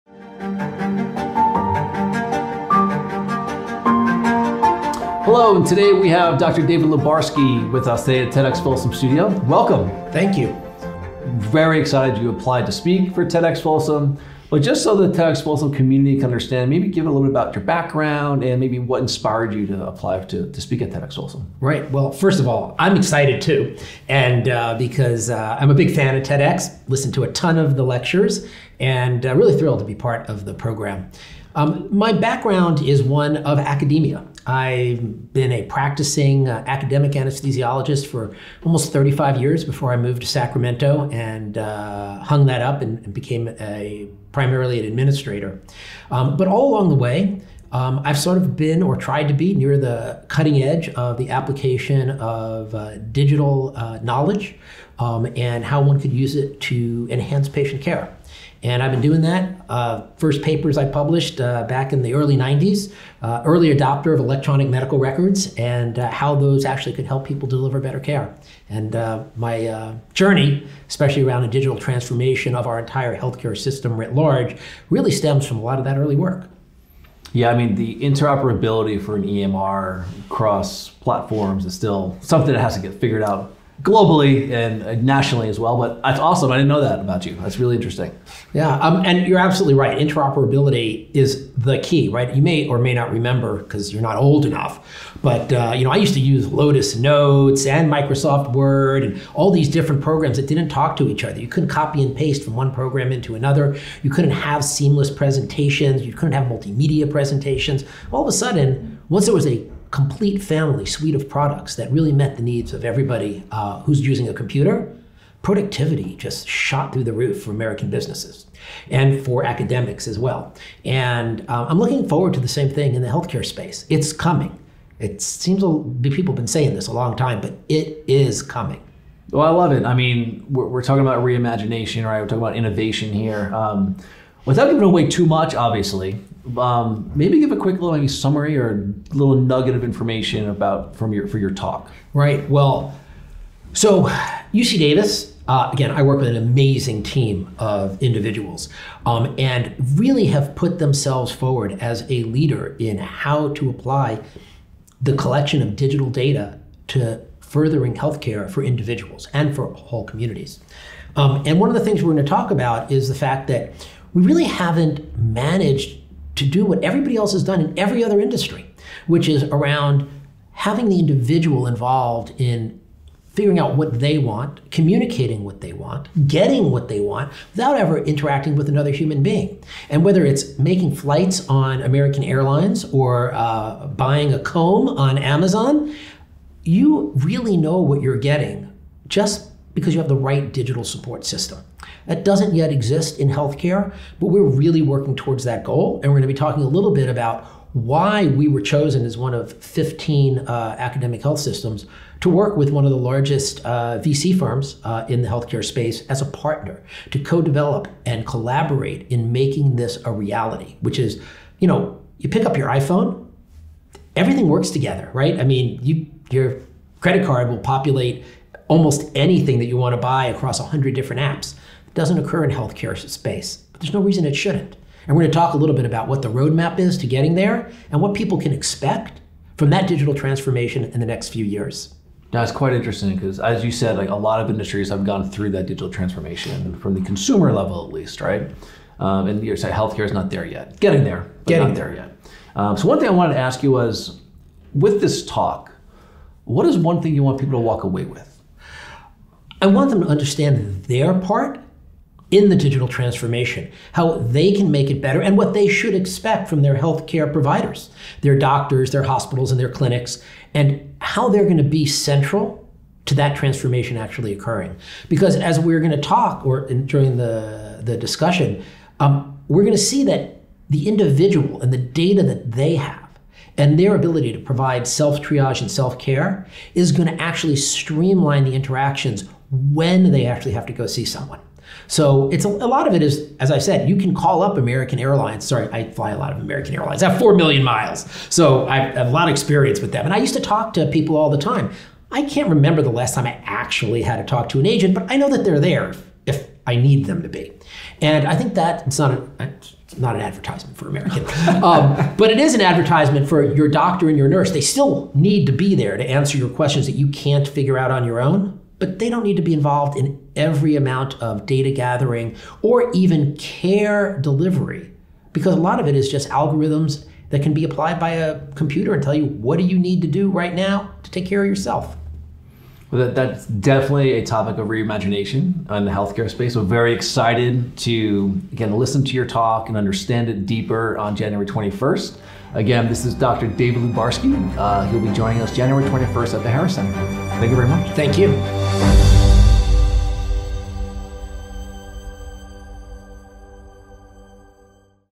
Dr. David Lubarsky - TEDxFolsom In Studio Discussion
In this video, TEDxFolsom Speaker Dr. David Lubarsky sits down with TEDxFolsom Executive Director Matthew Loughran, EMBA in the TEDxFolsom Studio at Granite City Coworking Loft in Historic Folsom to discuss his upcoming TEDxFolsom talk. tedx tedxtalk